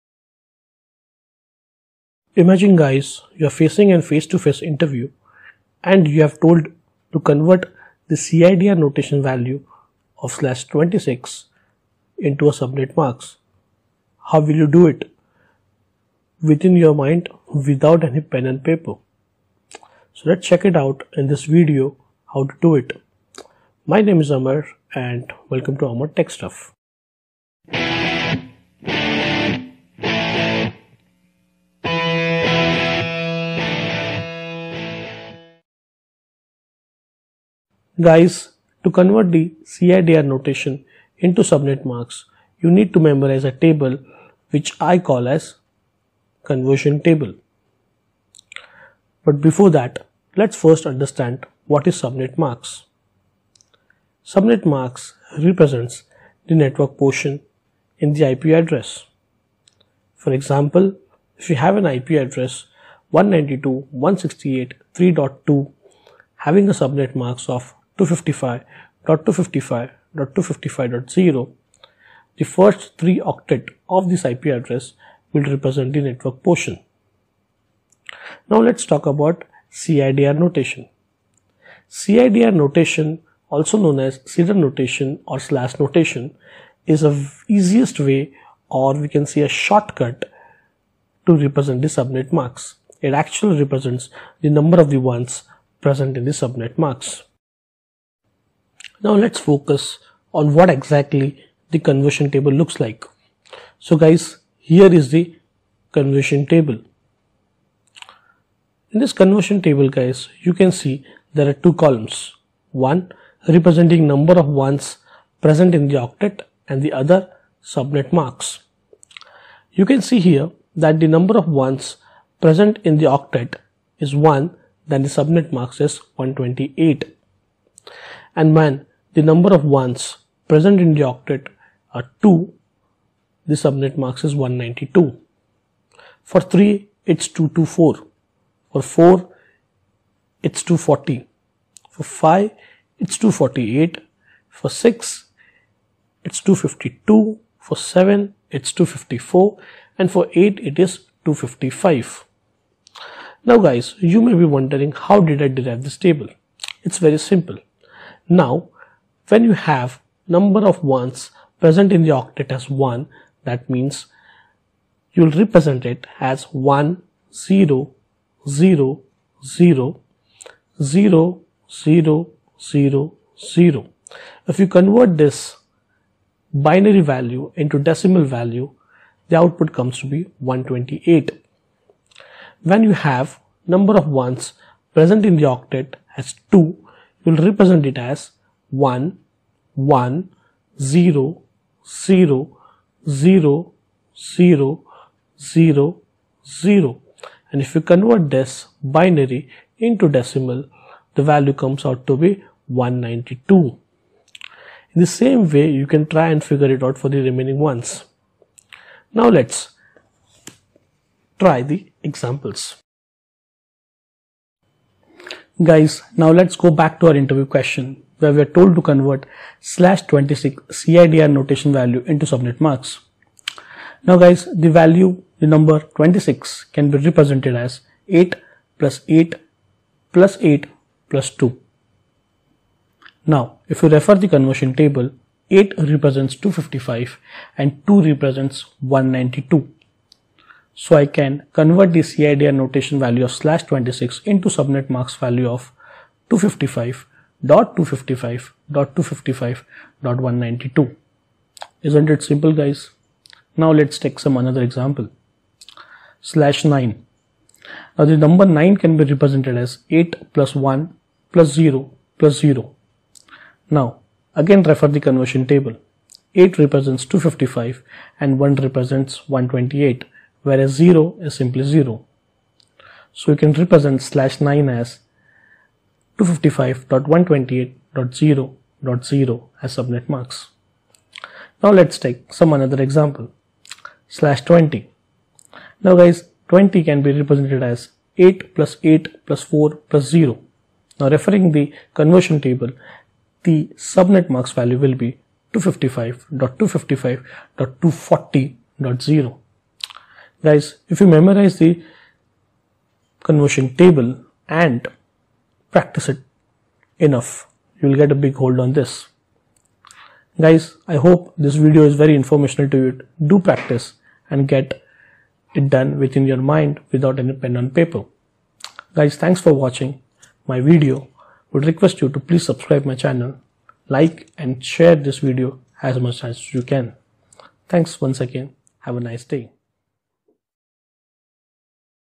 How to Convert CIDR Notation value into Subnet Mask | Animation Video
Calculate Subnet Mask from CIDR Notation

Hi Guys in this video we will focus on how to convert cidr notation value to subnet mask within our mind
without pen and paper .
To do so we need to memorize a conversion table and do some practice to get a good hold on same .

You can write me on -